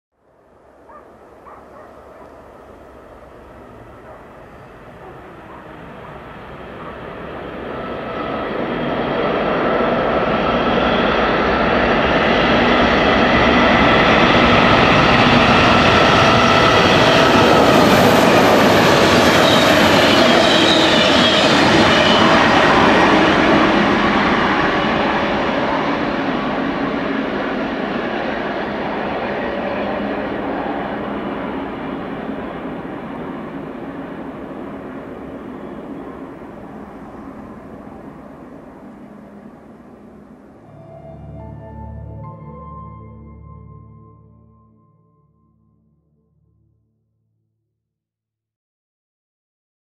DAS ist kurioser Nahverkehr! Eine 232 an Stuttgarter n- Wagen als Regionalexpress unterwegs!
Heute wäre so etwas gar nicht mehr möglich, die Wahl wären Schienenersatzverkehr mit Bus oder gleich Ausfall.
Als zu Beginn des Jahres 2011 drei 612 auf einem Baum landeten und damit
vorerst nicht einsatzfähig waren, musste Ersatz her.
Was nehmen? Triebwagen waren nicht überzählig vorhanden- aber es gab noch genug Lokführer mit 232 Berechtigung! So wurde eine Lok von DB Cargo angemietet und man holte aus Stuttgart noch drei n-Wagen heran- in Erfurt gab es längst keine mehr für ein zusätzliches Angebot.
Der Steuerwagen konnte natürlich nicht mit der 232 genutzt werden, die Lok setzte an den Zielbahnhöfen Altenburg und Erfurt einfach klassisch um.
Im Video saust 232 209 das Mühltal hinter Jena hinauf- es geht vom Saaletal auf die Hochebene nach Großschwabhausen. Mit den drei Wägelchen hat die Lok kein Problem, die Jahre zuvor hier verkehrenden Interregios fuhren deutlich langsamer herauf.
Lokbespannte Nahverkehrszüge in Thüringen mit Dieselloks sind seitdem Geschichte, heute herrschen hier 612, 642 und 650.
27012011